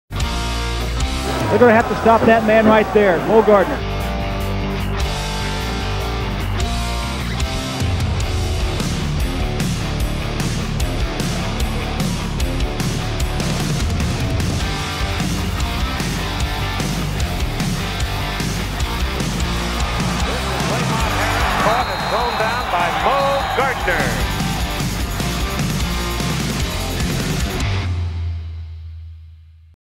Illini Football | Moe Gardner 2022 CFB Hall of Fame
Illinois legend Moe Gardner has been selected as a member of the College Football Hall of Fame Class of 2022, the National Football Foundation and College Football Hall of Fame announced Jan. 10 on ESPN. Gardner will join 17 previously selected Fighting Illini in the Hall of Fame, including college football legends Red Grange (inducted 1951), Robert Zuppke (inducted 1951) Dick Butkus (inducted 1983), and most recently Dana Howard (inducted 2018).